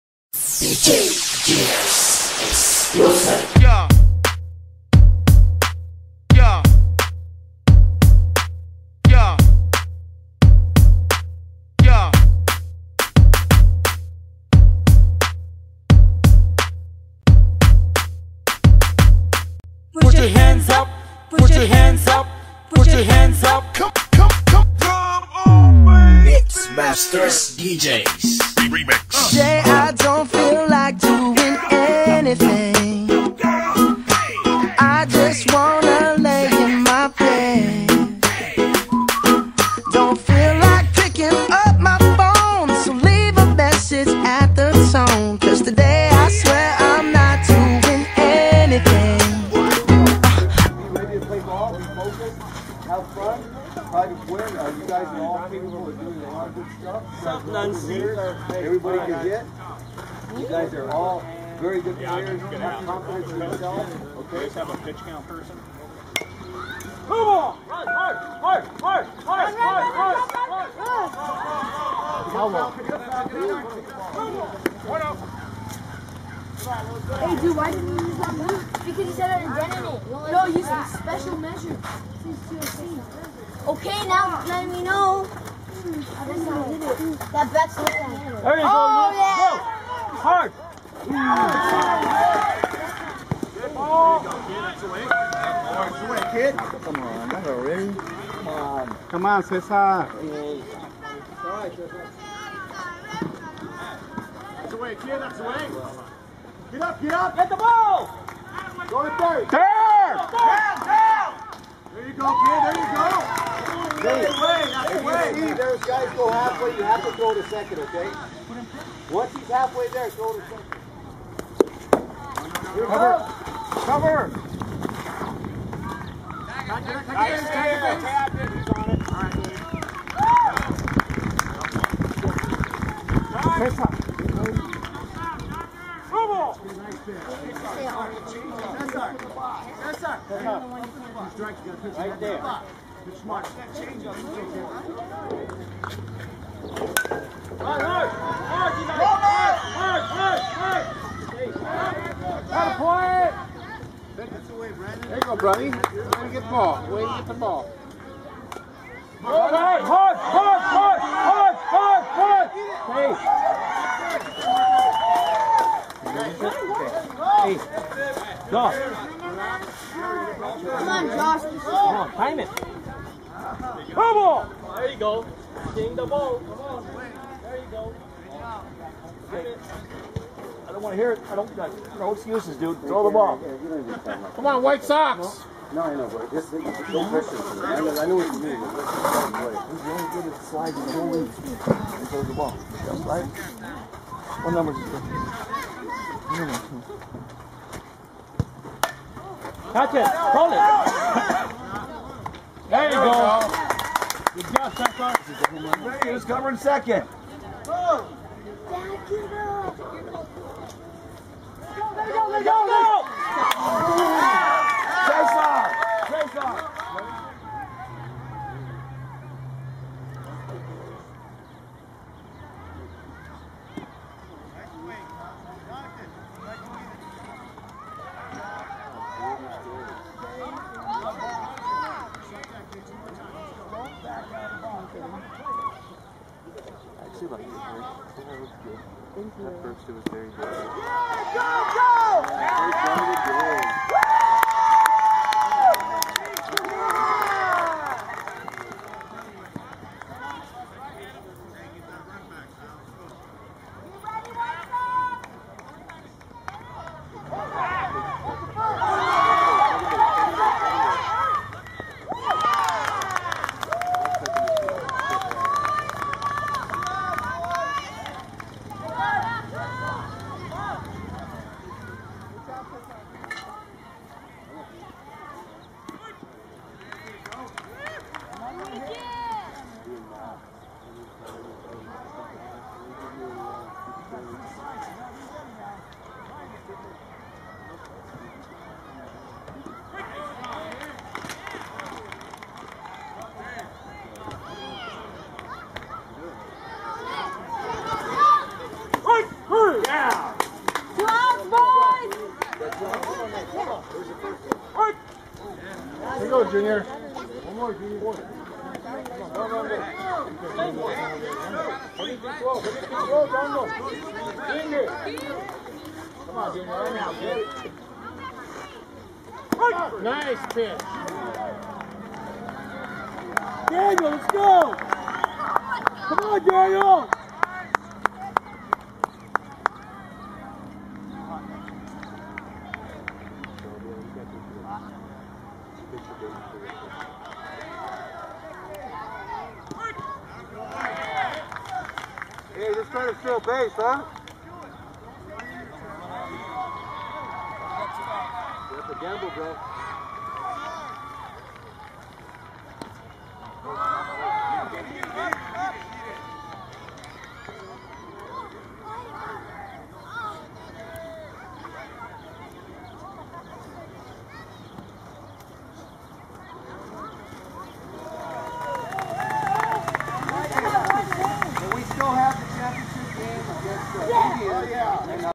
La Habra TOC White Sox V Dodgers 2011
La Habra Little League Coach Eric advances to TOC once again to try for the Second time in a row for TOC Championship.  Last year  the coach took the title of La Habra League League Champions & TOC Championship.  Didn't quite make it this year but the fact he advanced says it all!!! A humble Coach